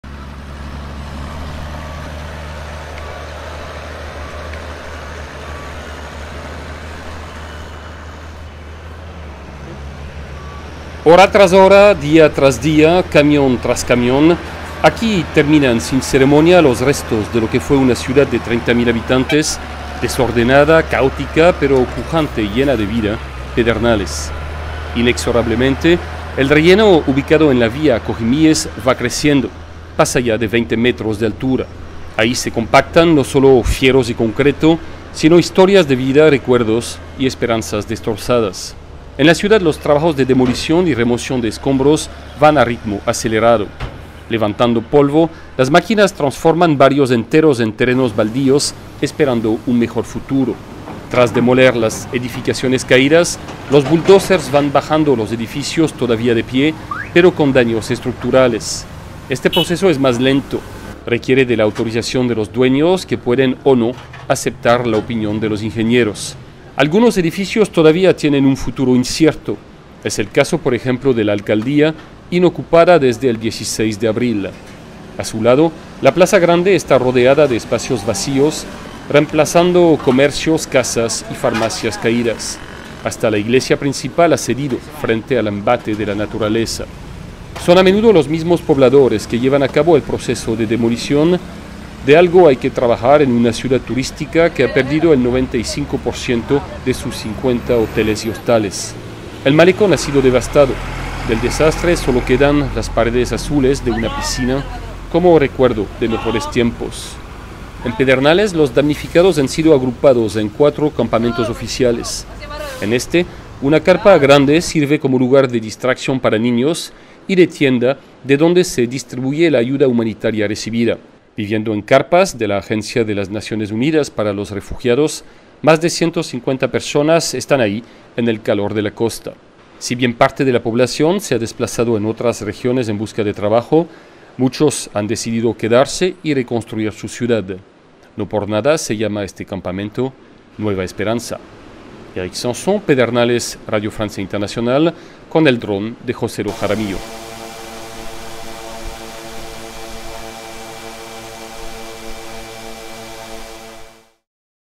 Pedernales desde el aire, un mes después del terremoto
A un mes del sismo que sacudió Ecuador, dejando 660 muertos, unas 30.000 personas siguen viviendo en campamentos en Pedernales. Un informe con imágenes aéreas de la ciudad tras el terremoto.
Informe: Eric Samson. Drone piloteado por Joselo Jaramillo.